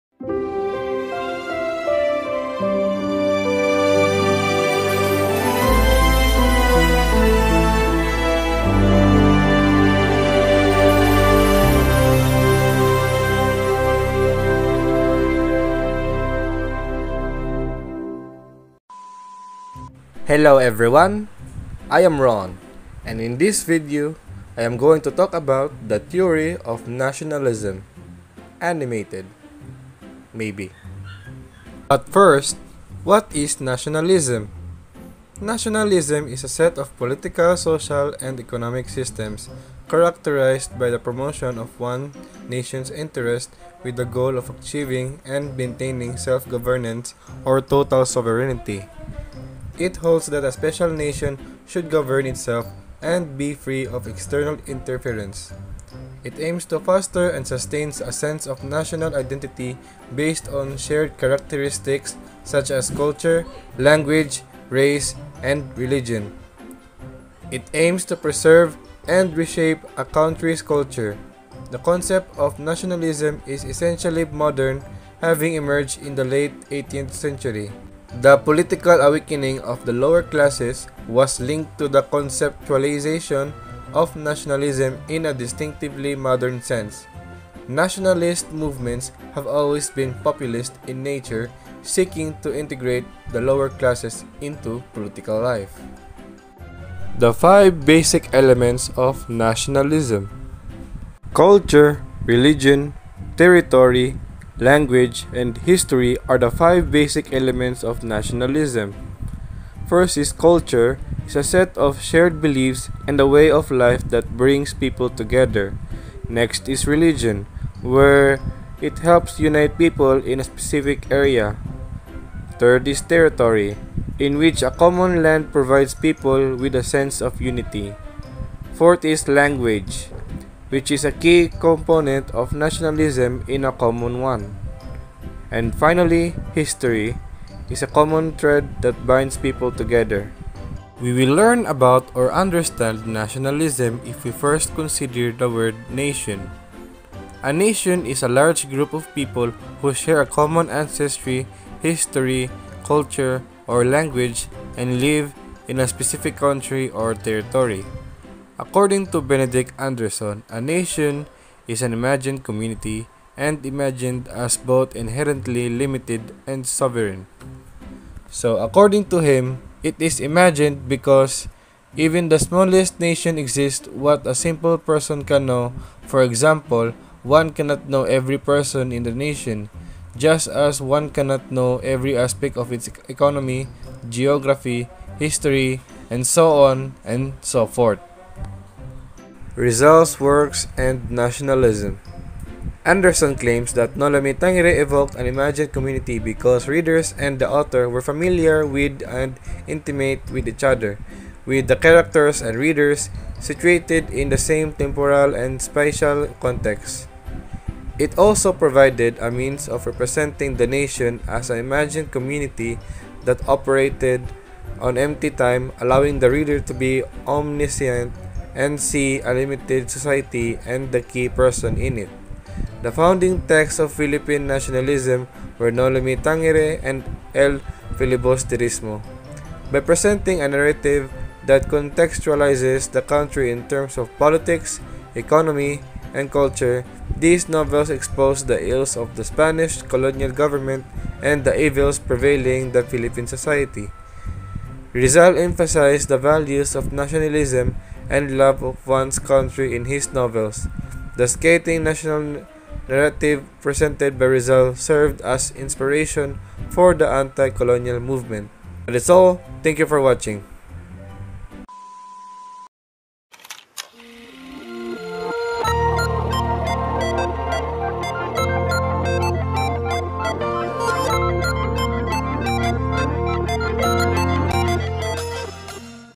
The Theory of Nationalism Animated (Maybe)
This video discusses the concept of nationalism in the context of the Philippines. This article examines the nation as a 'imagine community' in relation to Rizal and popular nationalism. It also explains the theory of nationalism in relation to the Rizal Law's enactment.

Subject: Rizal's Life and Works